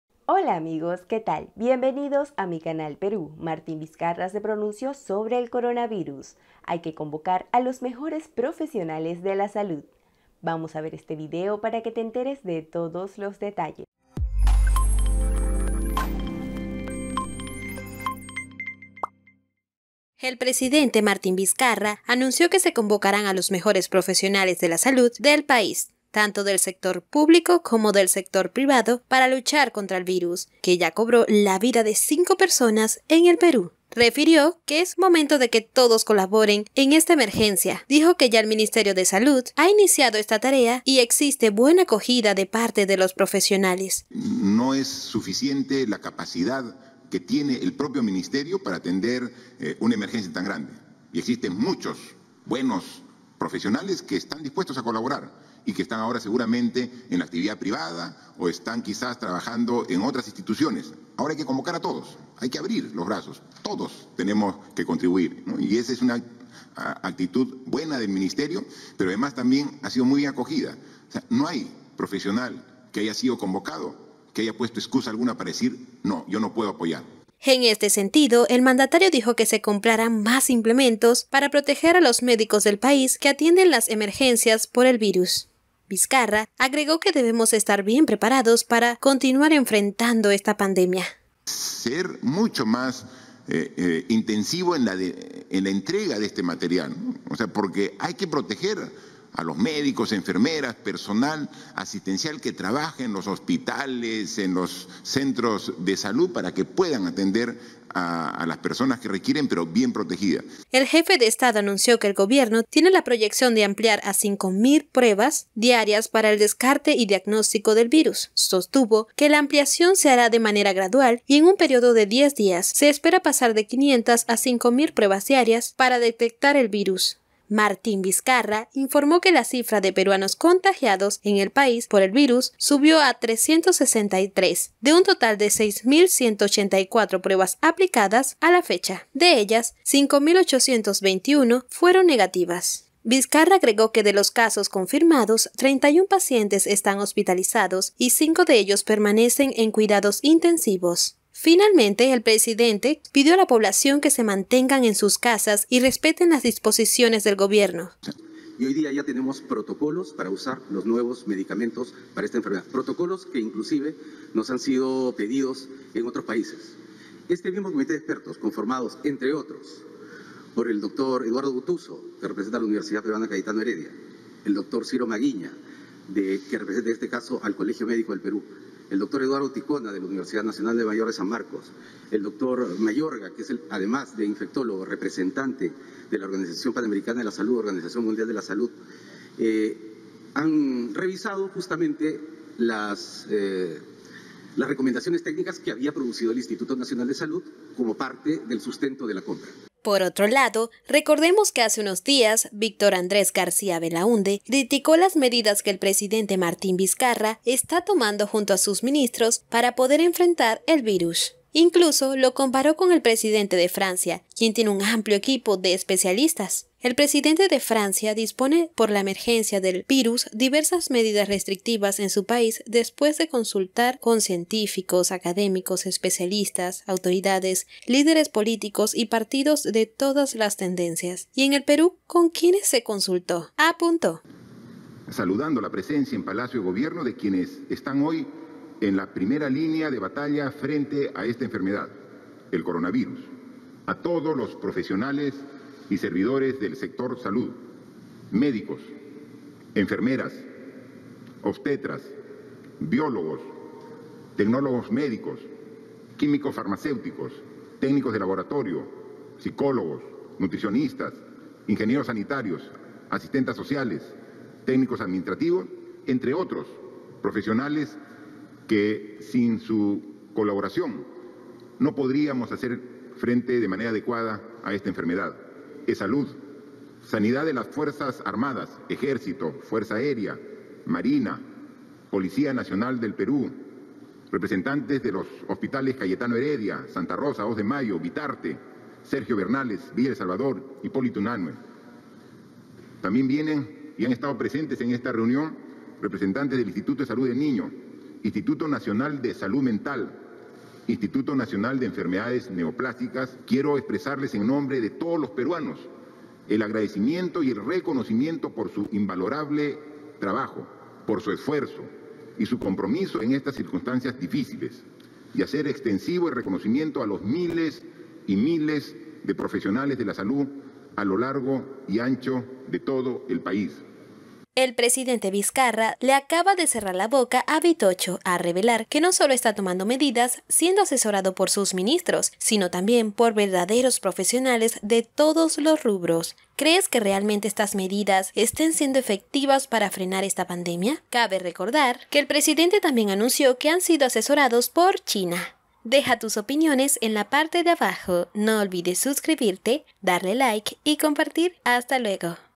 MARTIN VIZCARRA CUADRÓ A VITOCHO POR NINGUNEAR A LOS PROFESIONALES DE LA SALUD PERUANOS
Vitocho cuestionó a vizcarra y lo comparo con España e Italia, sin embargo el presidente y el ministro de salud le dio una contundente respuesta para que respete a los profesionales calificados de peru.